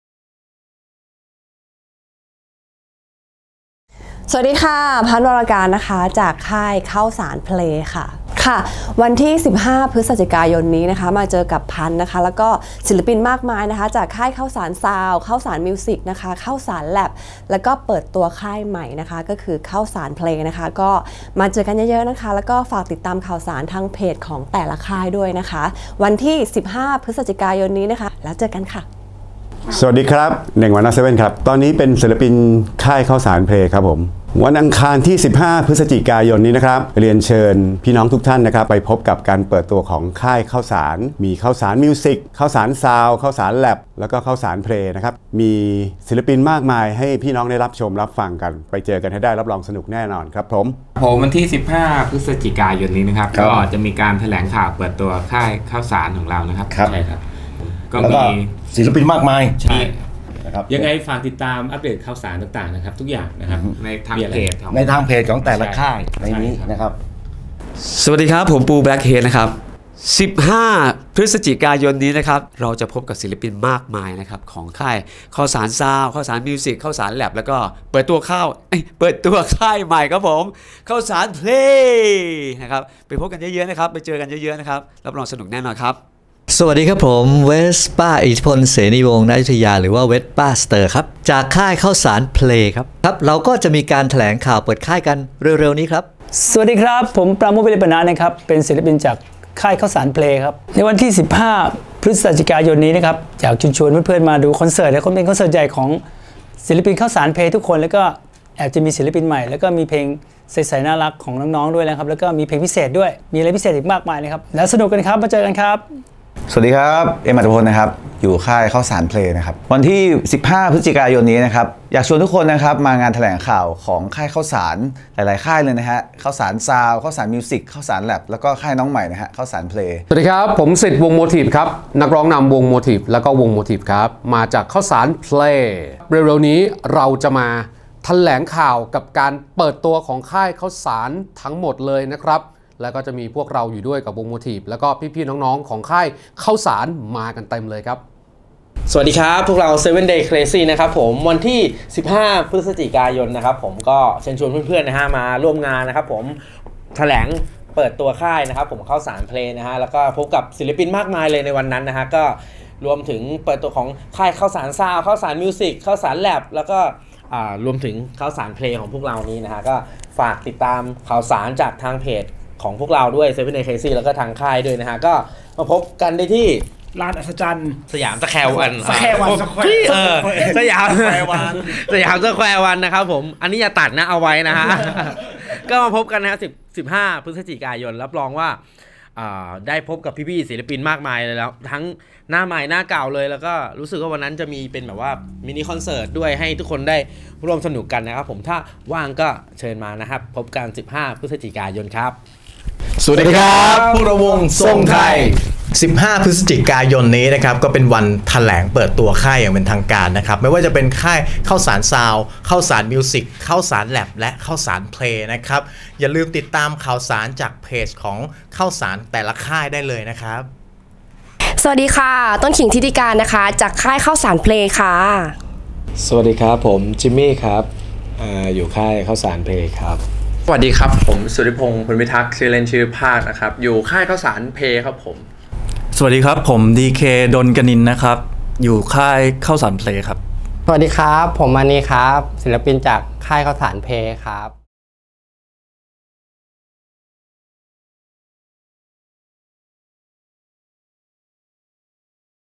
ข้าวสาร เอ็นเตอร์เทนเม้นท์ กลับมาเปิดค่ายเพลงอีกครั้ง!
เตรียมจัดงานแถลงข่าวเปิดตัวค่ายในเครือและค่ายน้องใหม่
“KHAOSAN  PLAY” ในวันอังคารที่15พฤศจิกายน 2565 นี้
ที่สยามสแควร์วัน เวลา 16.00น. เป็นต้นไป
ในงานพบกับศิลปินมากมายจาก….ค่าย
-Khaosan Music
-Khaosan Sound
-Khaosan Lab
-Ks Gang
Punch พั้นช์ วรกาญจน์ NY7 NengYnot7 ทรงไทย (SongThai) Smile Buffalo เวสป้า ster ปราโมทย์ วิเลปะนะ 7Days Crazy Auttapon Prakopkong  Blackheadpoo Charlie Potjes